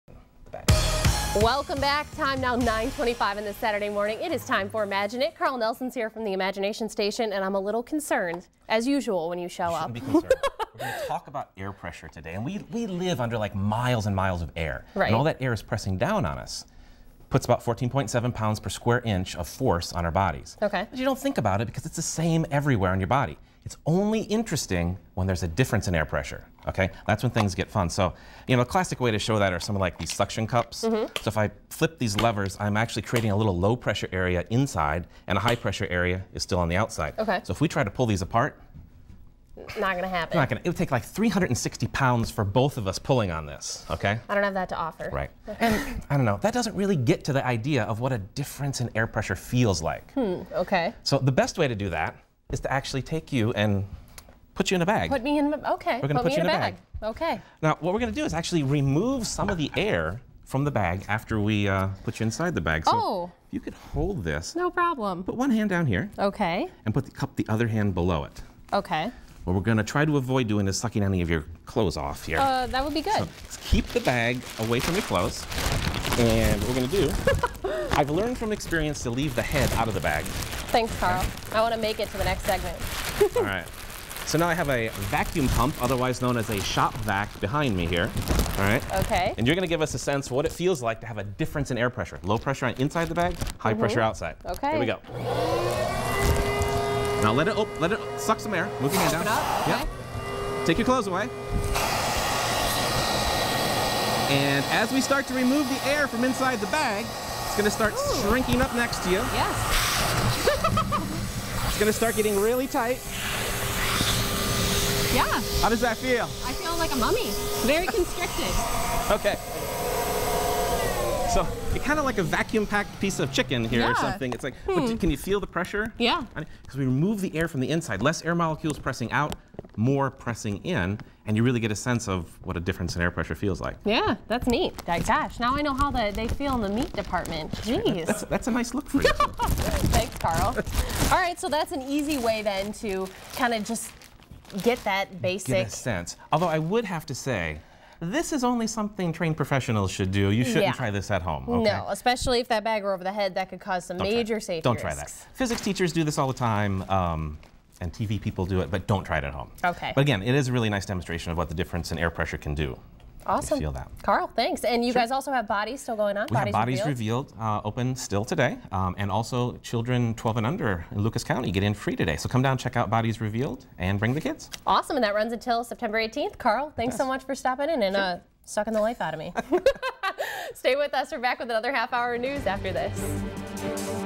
How to Vacuum Pack a Person
Chief Scientist Carl Nelson teaches us about differences in air pressure!

We live in a ocean of air, in fact, we live at the bottom of that ocean of air. All those miles of air above us end up exerting a force of about 14.7 pounds over every square inch of our bodies. We take it for granted since the force per area (pressure) is the same all around us.

Things start to get interesting when there is a slight imbalance in that pressure. What better way to find out what an imbalance in pressure feels like than to vacuum-pack WTVG-13 weekend anchor Christina Williams?